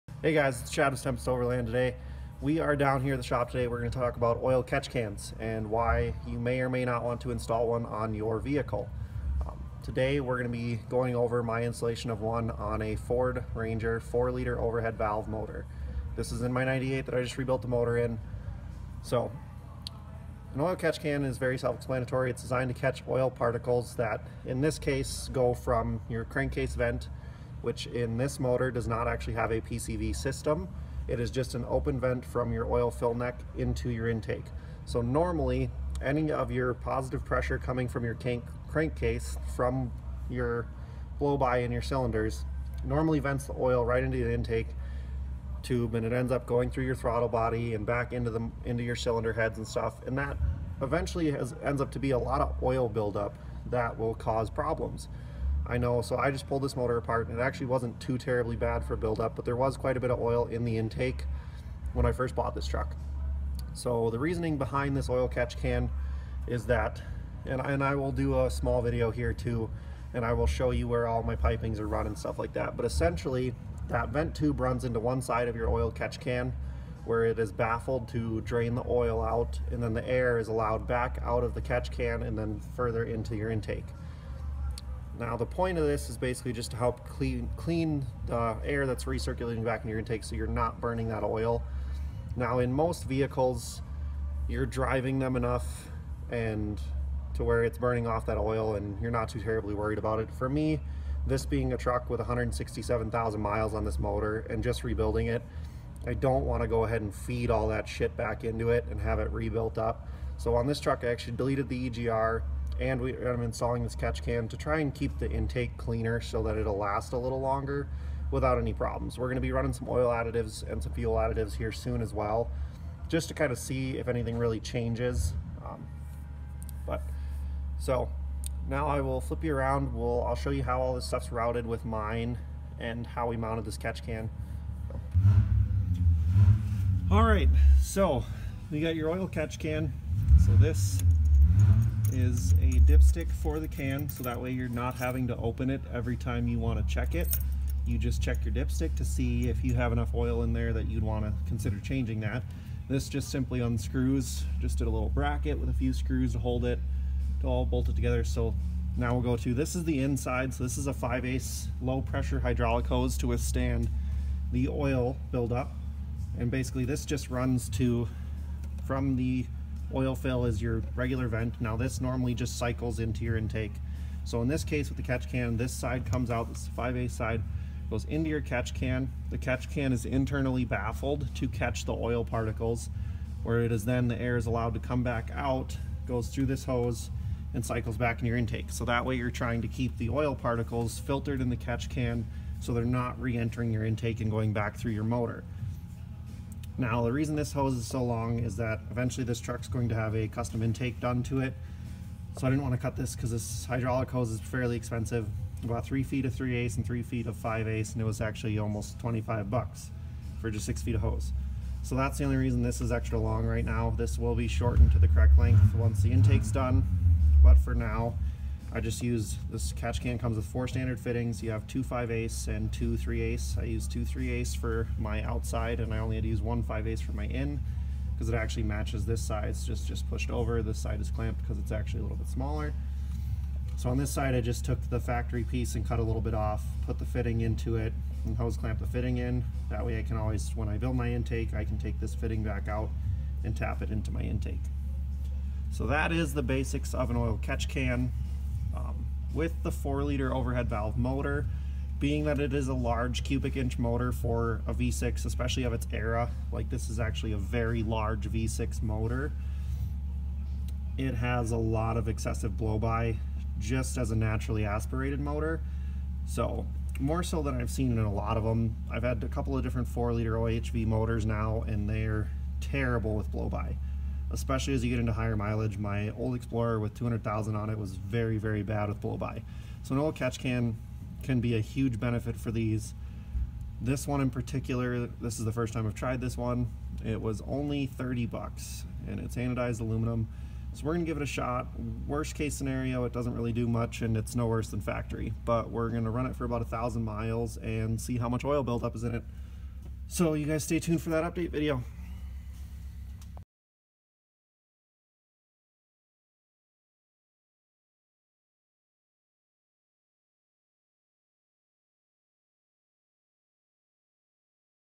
Ford Ranger Oil Catch Can
My 1998 ford ranger 4.0 OHV Oil Catch Can install, and why you might want one.